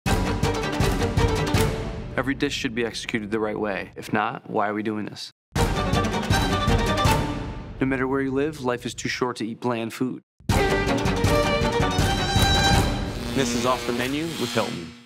The perfect homemade pasta dish! 🍝
This pasta recipe (perfected by Red House Tavern’s executive chef Joel Valente) combines homemade pasta and a velvety lamb ragu that no one can resist.

Read more about chef Joel Valente and his passion for his craft.

Visit Red House Tavern for locally sourced traditional dishes with a modern twist.